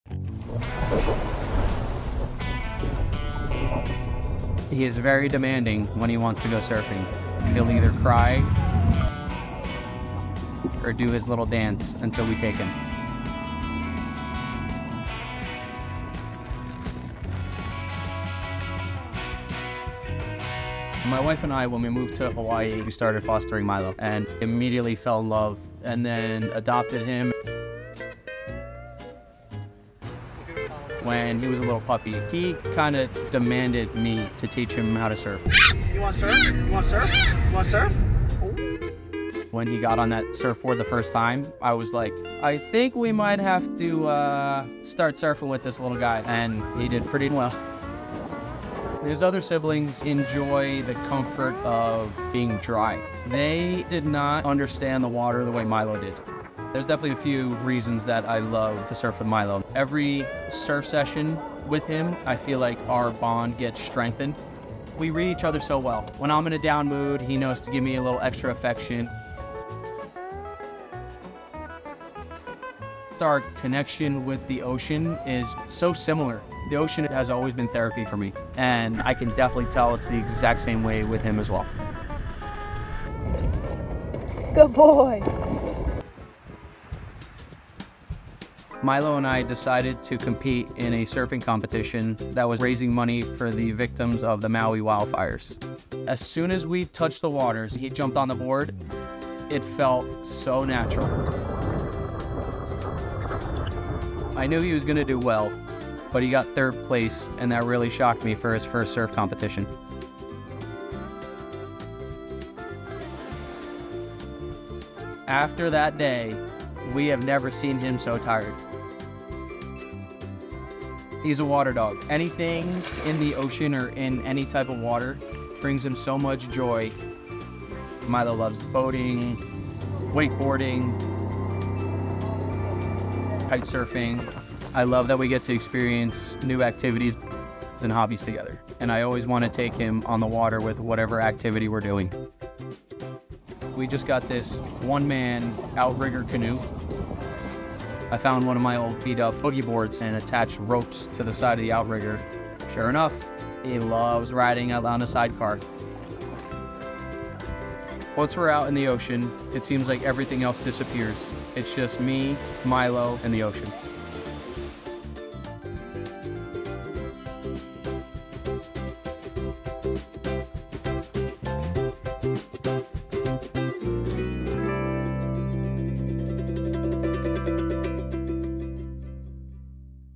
This Dog Has Demanded To Surf Since He Was A Baby | The Dodo
Puppy does the cutest dance to beg his dad to go surfing — and then he learns to kite surf! 🌊

Help the people and animals of Maui recover from the wildfires by donating to the Maui Strong Fund & Maui Humane Society